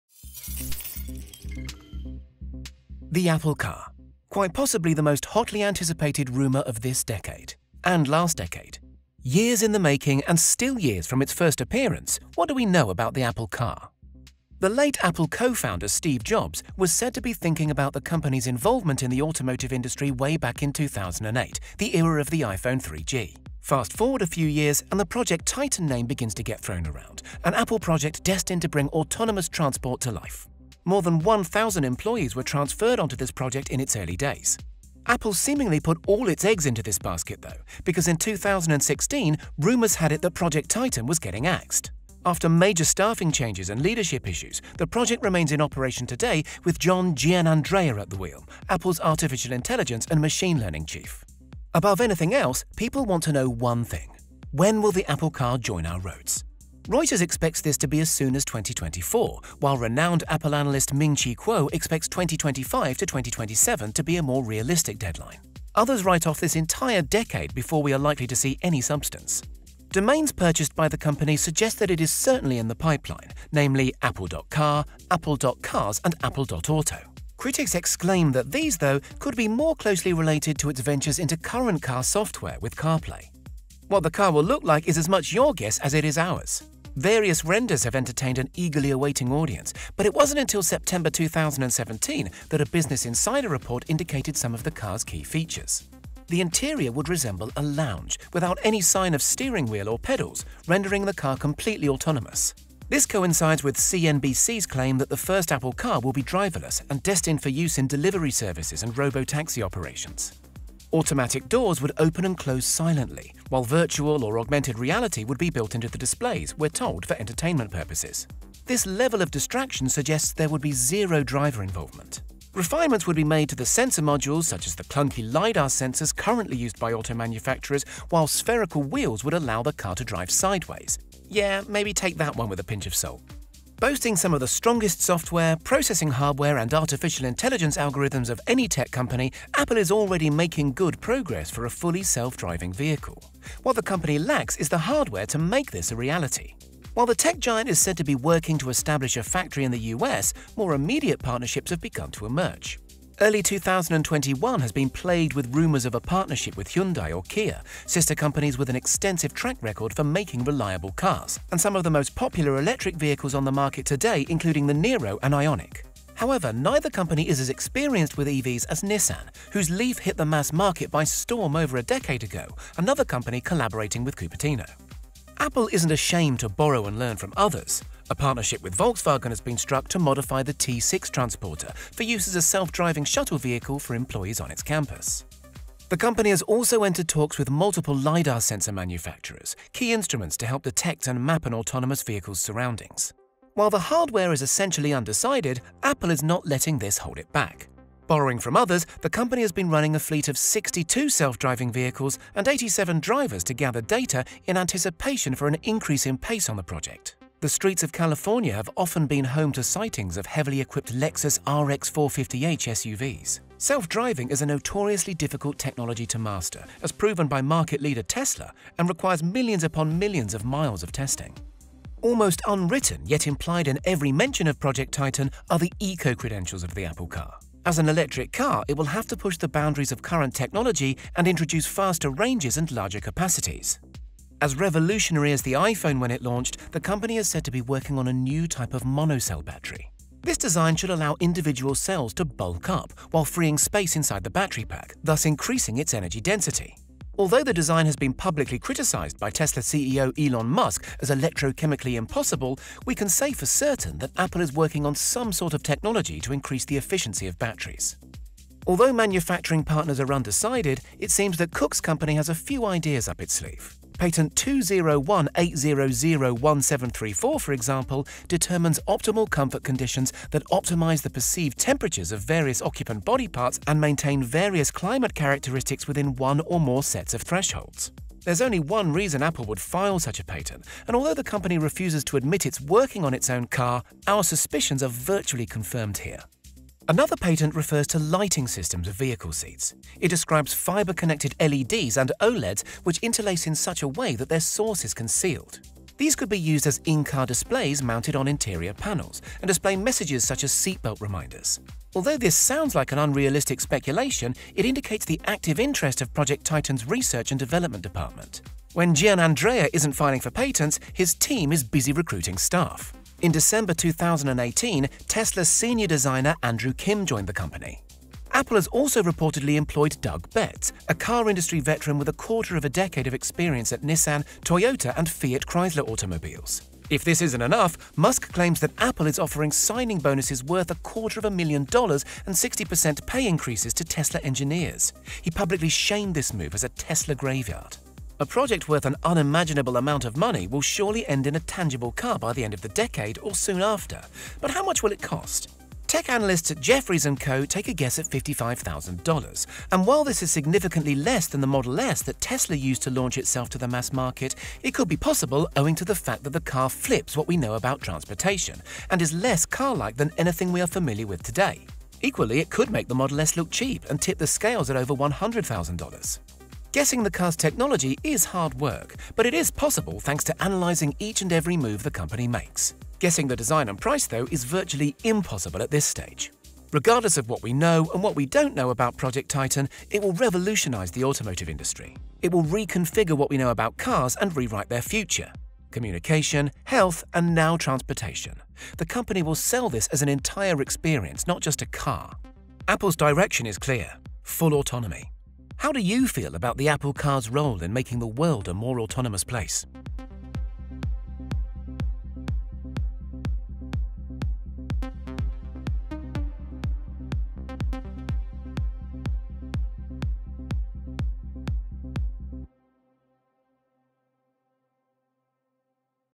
The Apple Car Is Coming Sooner Than You Think
The Apple Car. Quite possibly the most hotly anticipated rumour of this decade. And last decade. Years in the making, and still years from its first appearance, what do we know about the Apple Car?

The late Apple co-founder Steve Jobs was said to be thinking about the company’s involvement in the automotive industry way back in 2008, the era of the iPhone 3G. Fast forward a few years and the Project Titan name begins to get thrown around, an Apple project destined to bring autonomous transport to life. More than 1,000 employees were transferred onto this project in its early days.

Apple seemingly put all its eggs into this basket though, because in 2016 rumours had it that Project Titan was getting axed. After major staffing changes and leadership issues, the Project remains in operation today with John Giannandrea at the wheel - Apple’s artificial intelligence and machine learning chief.

Imagery supplied via Getty Images

Apple Car